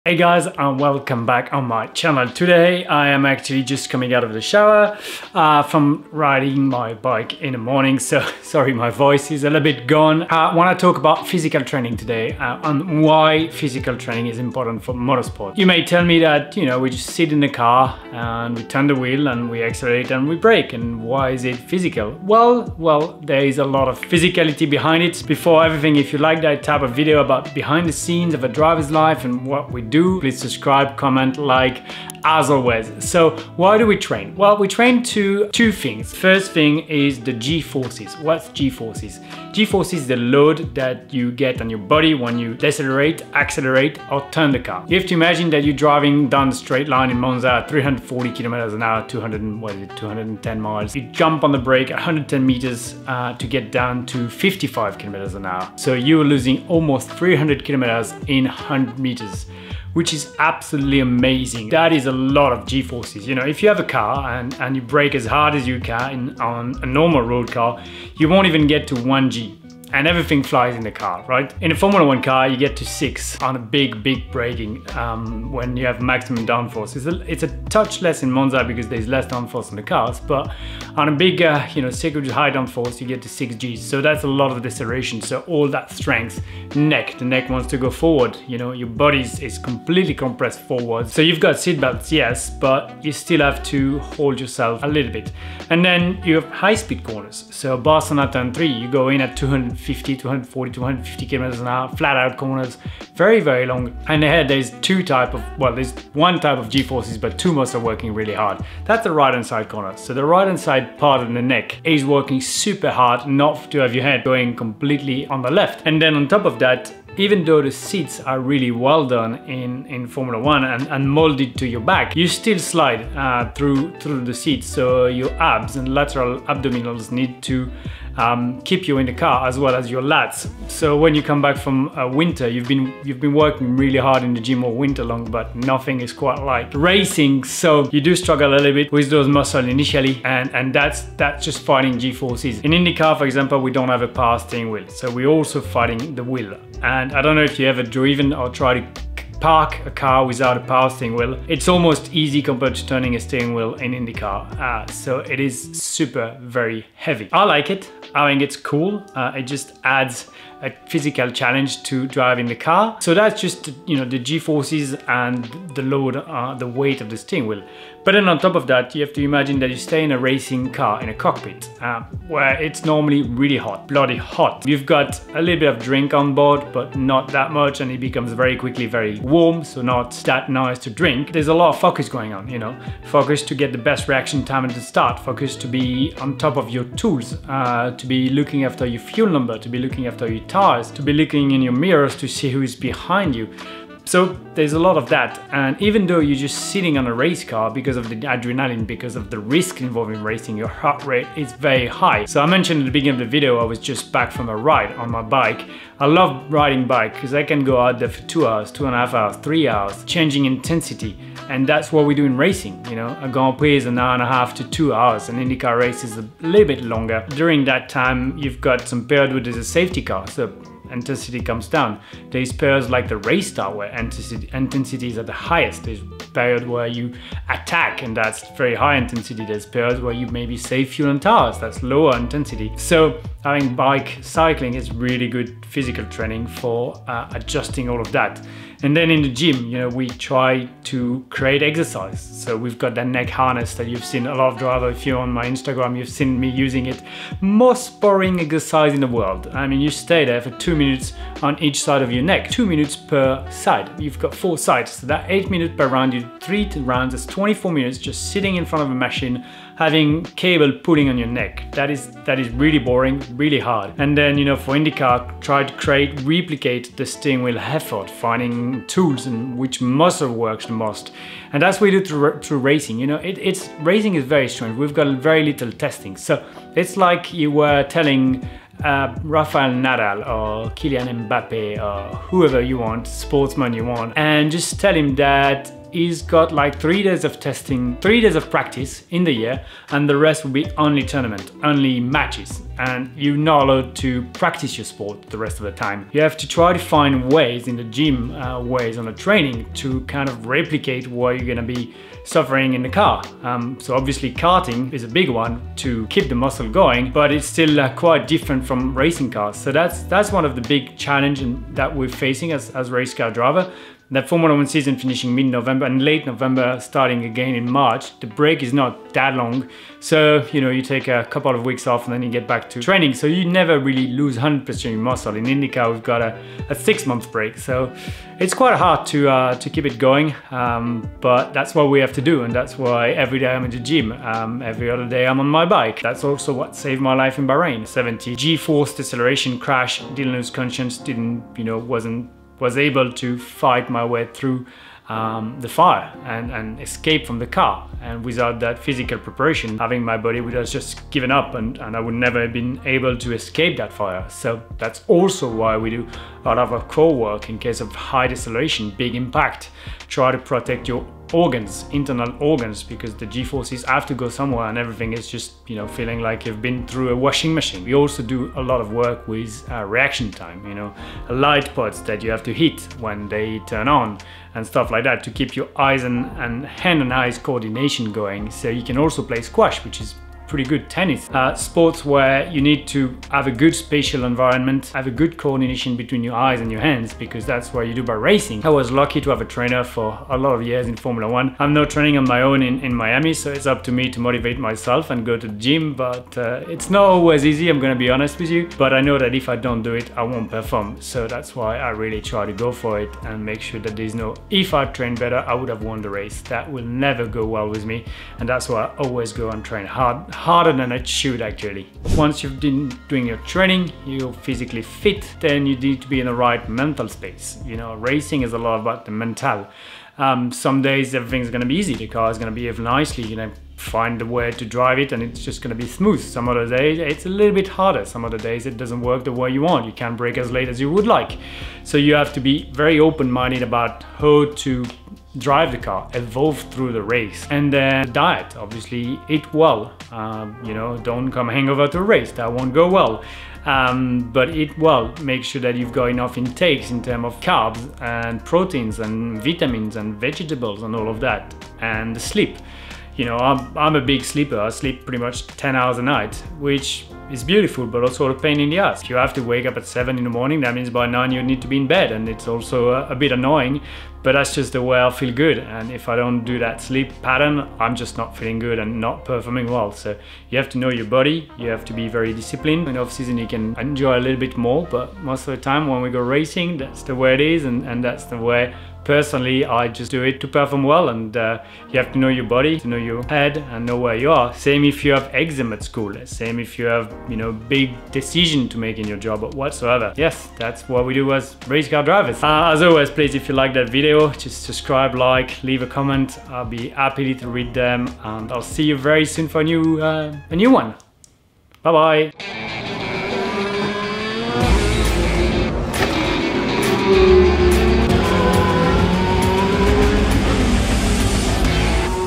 Pro Drivers Physical Preparation for Extreme Motorsport | Romain Grosjean
Hey Guys !
Training is a key to success in motorsport and I wanted to explain you my routine.
As you know in Formula 1 and Indycar we have to undergo extreme conditions and exhausting races. That's why we work out with specific exercises.